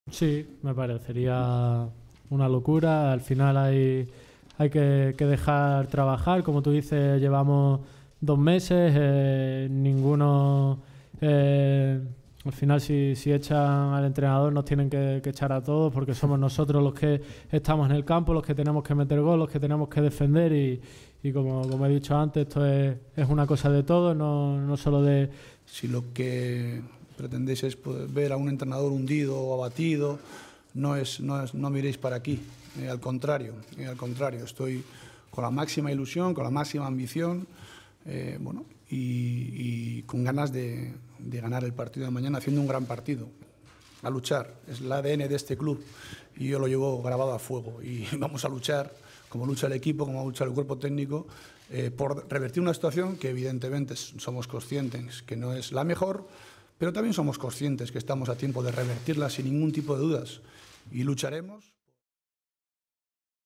If Madrid sack Lopetegui they should sack us all, says Isco
Real Madrid coach Julen Lopetegui has brushed off media reports that he could soon lose his job saying that there is still time to reverse the team's fortunes. Lopetegui has been at the helm for nine La Liga games, only four of which ended in victory for the club.
'If you're wanting to see a wounded coach, don't look over here,' the former Spain boss told a news conference before their Champions League tie with Viktoria Plzen on Tuesday. Meanwhile, midfielder Isco was firmly behind his manager, saying the players must take responsibility for the reigning European champions' slump.